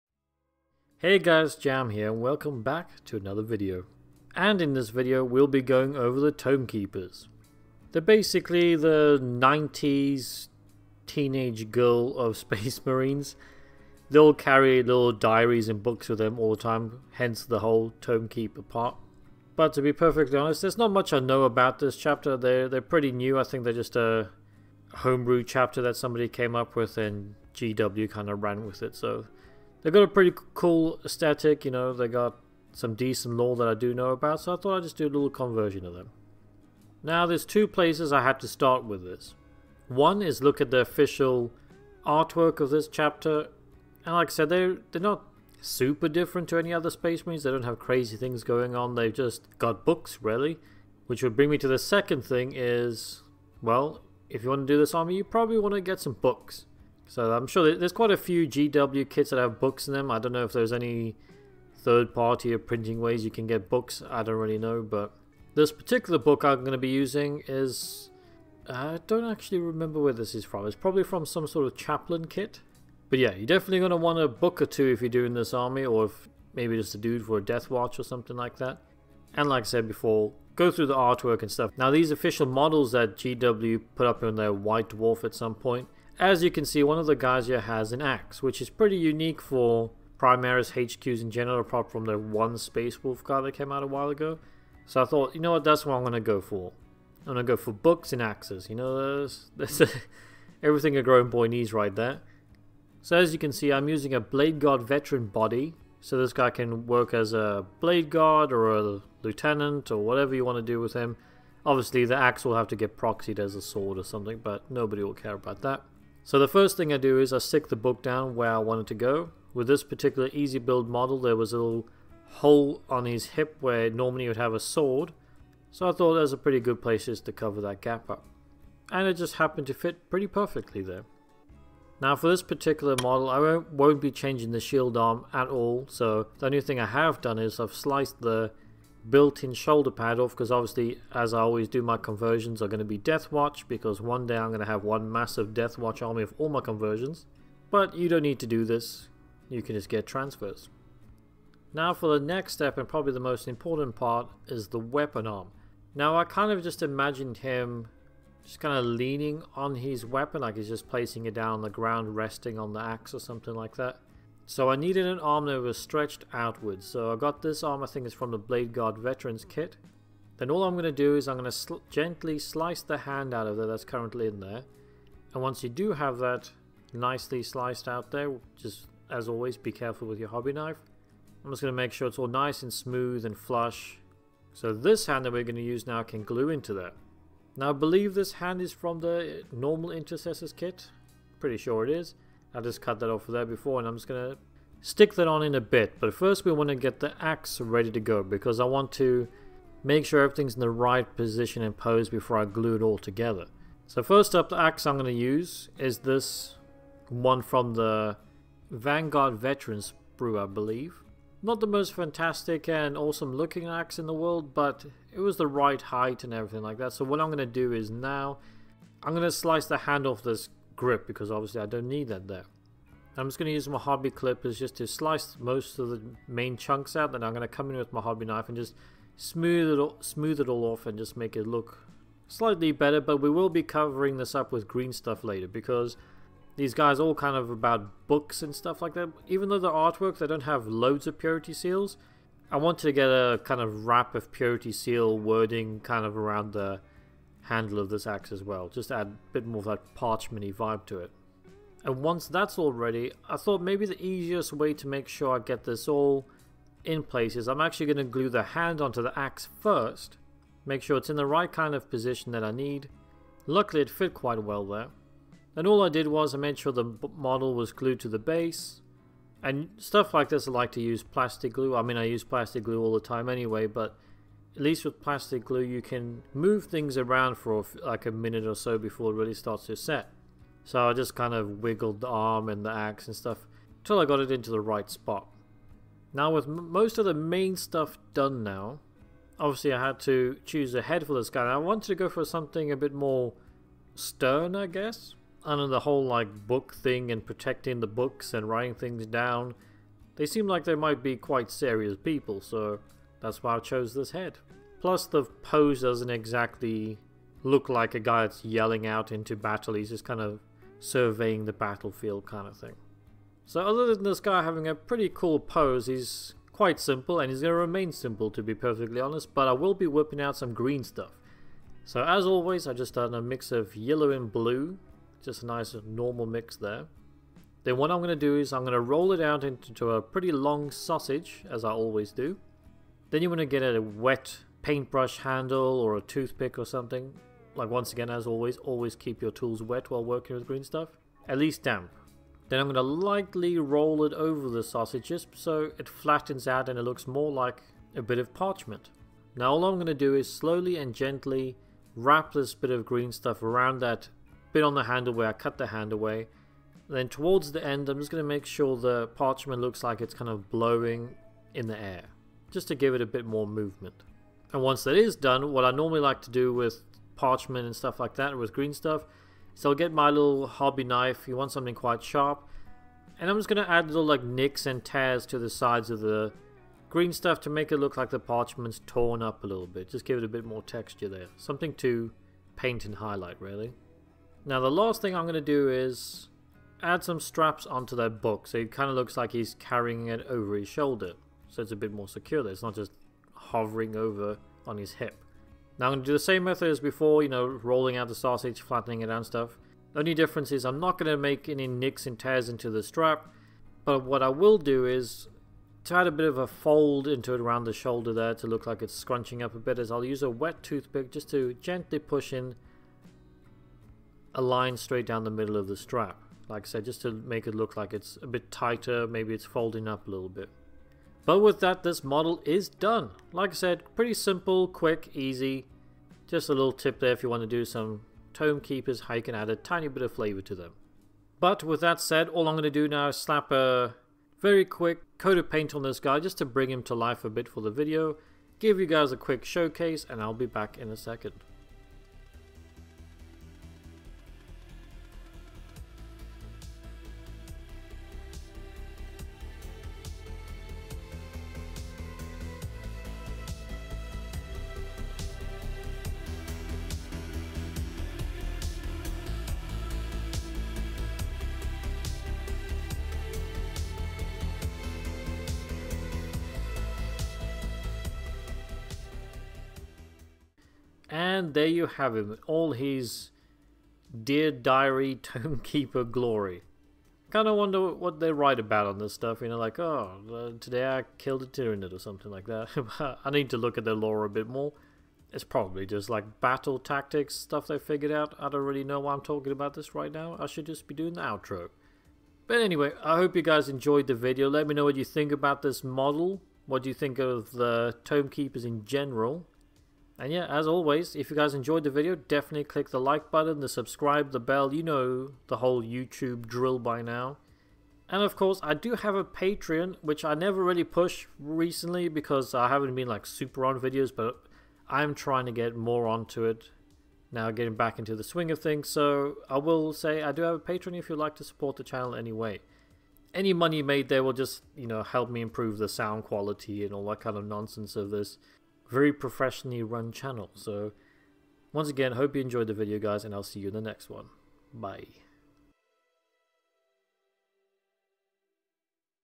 Kitbashing Tome Keepers
Hey everyone! In this video I'll be going through some light kitbashing / converting ideas and techniques for you to add that Tome Keepers flavour to all your Space Marines. This Chapter as the potential to look really cool if you can get hold of all the bits and pieces you'd need to give them that unique aesthetic.